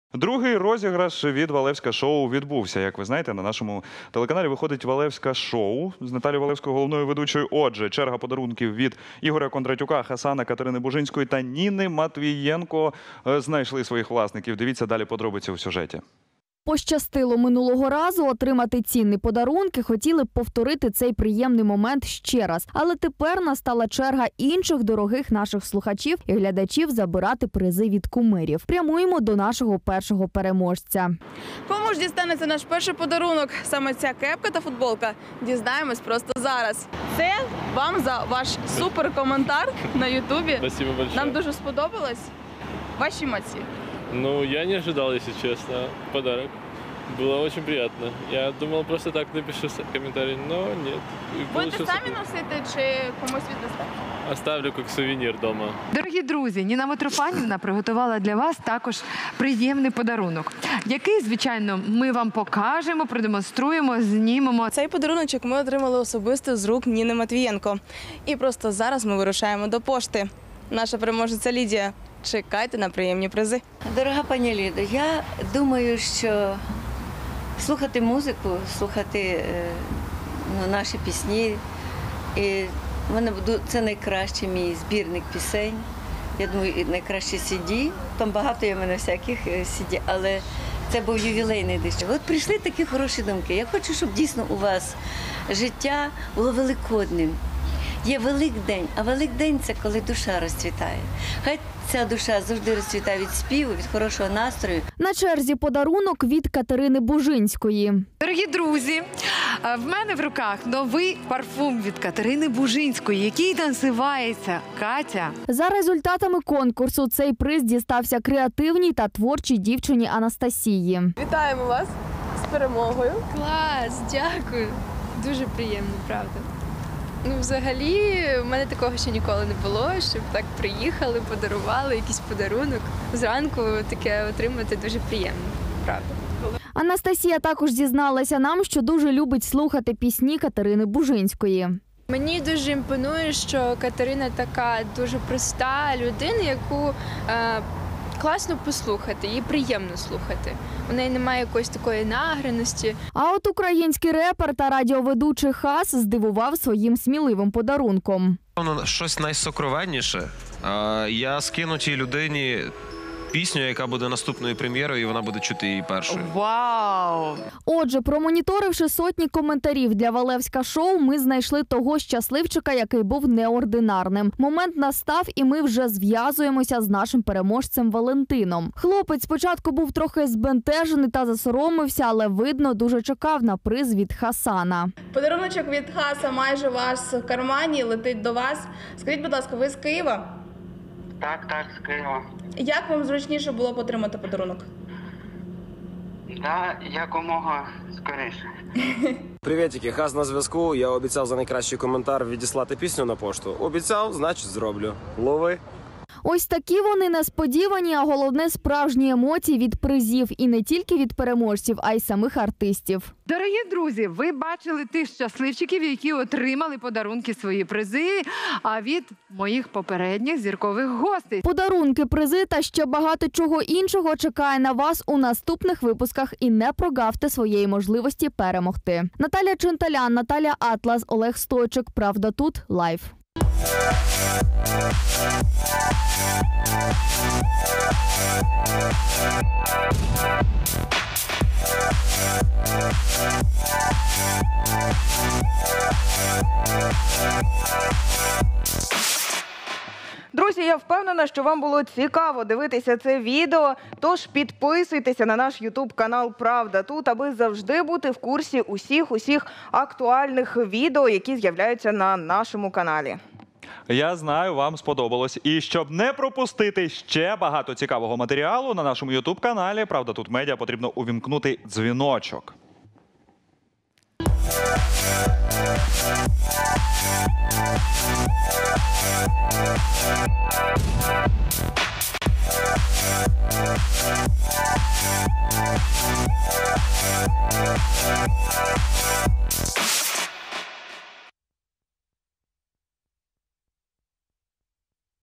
Другий розіграш від зіркових гостей «ValevskaShow» проведено!
Другий розіграш від зіркових гостей  «ValevskaShow» проведено! Черга подаруночків від Ігора Кондратюка, ХАСАНА, Катерини Бужинської  та Ніни Матвієнко. Сьогодні ми покажемо вам справжні емоції, які нам вдалося  відчути і побачити від наших дорогих глядачів.
Ми впевнені, що тим кому пощастило минулого разу отримати цінні подаруночки, хотіли б повторити цей приємний момент ще раз. Але тепер настала черга інших, дорогих наших слухачів і  глядачів забирати призи від кумирів.
Подарунки, призи та ще багато чого іншого, чекайте у наступних випусках і не прогавте своєї можливості перемогти.